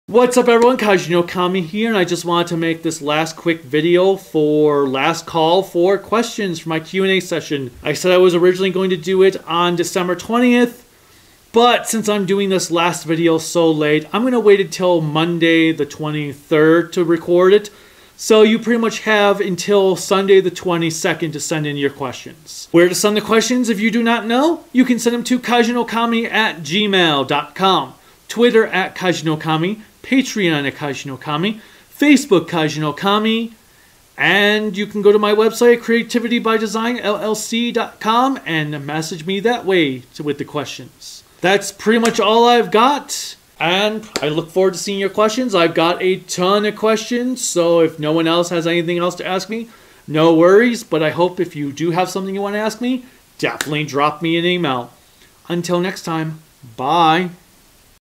Last Call for Questions
Last call to send me questions for my Q&A video on 12/23.

I also have my own Rightstuf sharesale link too